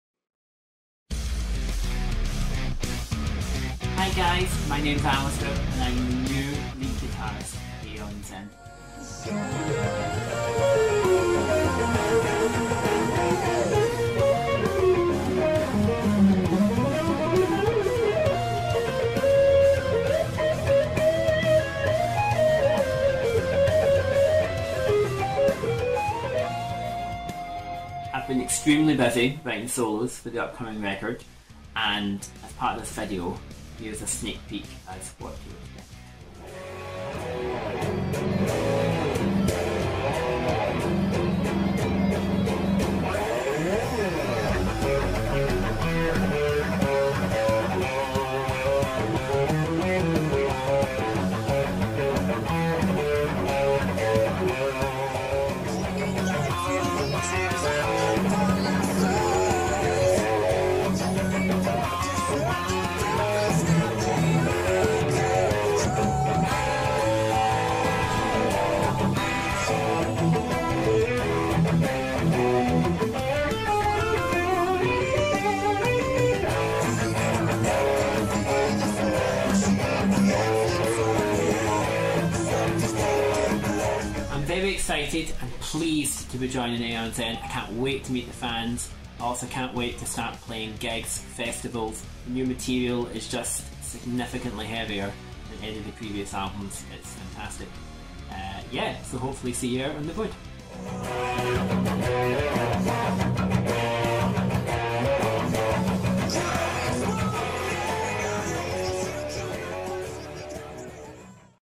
New Aeon Zen Lead Guitarist Announcement
AEON ZEN announces new guitarist

AEON ZEN are excited to announce the introduction of a new band member into their ranks: Alistair Bell.
Alistair takes over the lead guitar position from Matt Shepherd, who has left the band on good terms after making the decision to focus on his family life.

"Hi guys, my name's Alistair Bell and I'm the new lead guitarist in Aeon Zen.
I've been extremely busy writing solos for the upcoming record and as a part of this video, here's a sneak peak as what to expect.
I'm very excited and pleased to be joining Aeon Zen. I can't wait to meet the fans, and also can't wait to start playing gigs and festivals. The new material is significantly heavier than any of the previous albums, it's fantastic. So we'll hopefully see you out on the road!"

Aeon Zen are currently hard at work on their next album, due for release later this year.